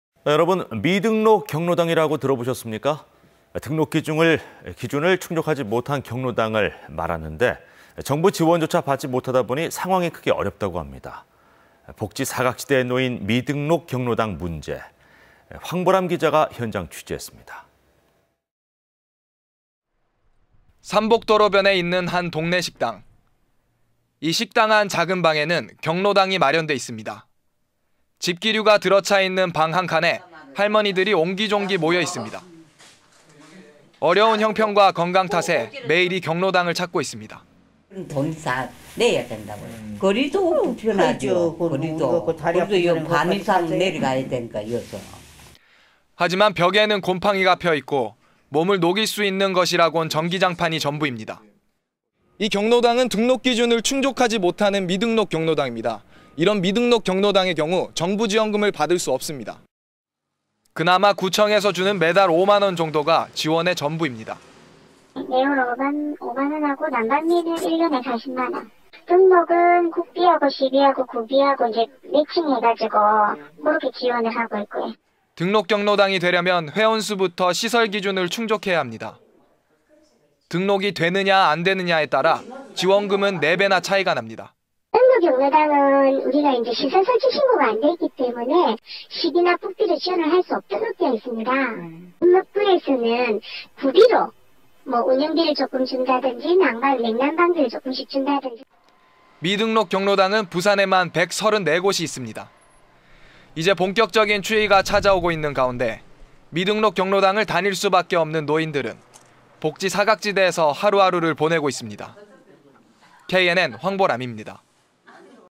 [KNN 뉴스]복지 사각지대 미등록 경로당
설명{앵커:
시청자 여러분 미등록 경로당이라고 이야기 들어보셨습니까?

등록기준을 충족하지 못한 경로당을 말하는데,
정부 지원조차 받지 못하다 보니 상황이 크게 어렵다고 합니다.

복지 사각지대에 놓인 미등록 경로당의 문제를,
황보 람 기자가 현장취재했습니다.}

{리포트}
산복도로변에 있는 한 동네 식당.

{수퍼:식당 안 작은 방에 마련된 경로당}
이 식당 안 작은 방에는 경로당이
마련돼 있습니다.

집기류가 들어차 있는 방 한 칸에
할머니들이 옹기종기 모여있습니다.

어려운 형편과 건강 탓에
매일 이 경로당을 찾고 있습니다.

{싱크:}
{수퍼:경로당을 찾은 할머니/"(다른 경로당은) 돈(회비)도 내야 하고, 거리도 멀어서 불편하죠. 여기서 반 이상을 걸어내려 가야해요."}

하지만 벽에는 곰팡이가 펴있고,
몸을 녹일 수 있는 것이라곤
전기장판이 전부입니다.

{StandUp}
{수퍼:황보 람}
"이 경로당은 등록 기준을 충족하지
못하는 미등록 경로당입니다.

이런 미등록 경로당의 경우,
정부 지원금은 받을 수 없습니다."

그나마 구청에서 주는
매달 5만원 정도가 지원의
전부입니다.

{싱크:}
{수퍼:구청 관계자/"매월 5만원하고, 난방비는 1년에 40만원. 등록(경로당)은 국비, 시비, 구비를 합쳐서 지원하고 (있습니다.)"}

등록 경로당이 되려면 회원수부터
시설 기준을 충족해야 합니다.

등록이 되느냐 안되는냐에 따라
지원금은 4배나 차이가 납니다.

{전화:}
{수퍼:부산시청 관계자/"미등록 경로당은 시설설치 신고가 안 돼있기 때문에 시비나 국비를 지원할 수 없도록 돼있습니다. 몇몇 구에서는 구비로 운영비나 냉난방비를 지원하고 있습니다."}

미등록 경로당은 부산에만 134곳이
있습니다.

{수퍼:영상취재 박동명}
이제 본격적인 추위가 찾아오고
있는 가운데 미등록 경로당을
다닐 수 밖에 없는 노인들은
복지 사각지대에서 하루 하루를
보내고 있습니다.

KNN 황보 람입니다.